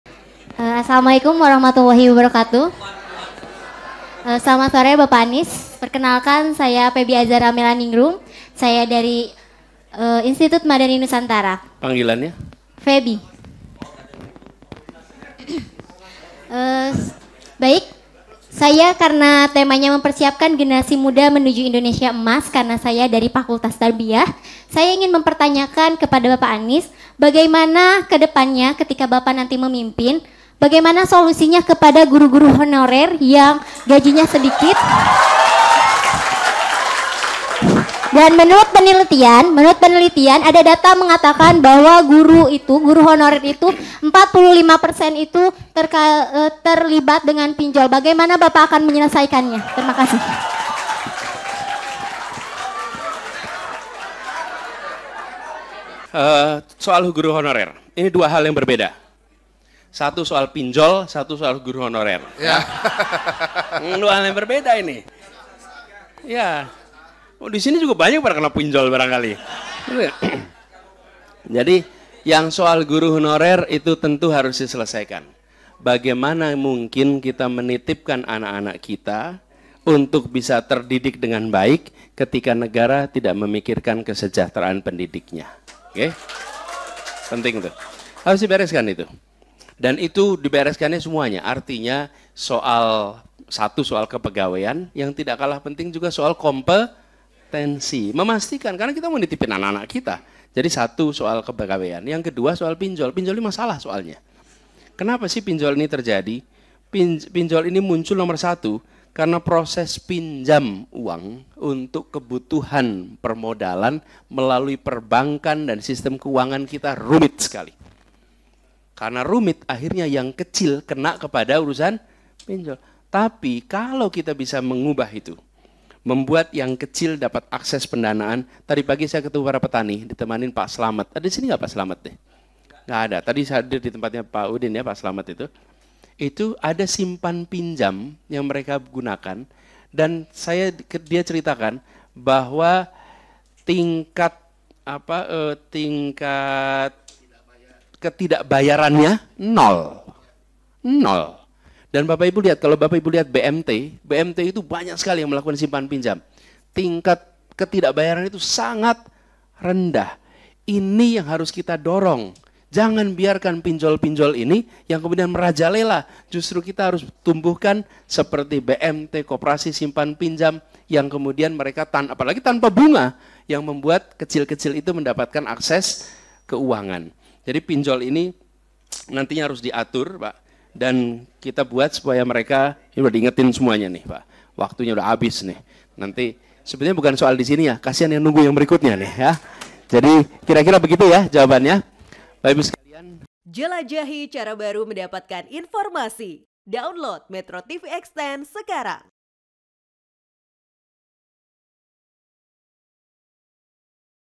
Guru Honorer Terlilit Pinjol, Anies Kasih Solusi!
MetroTV, Bakal Calon Presiden Anies Baswedan memberikan Dialog Kebangsaan di IMIN.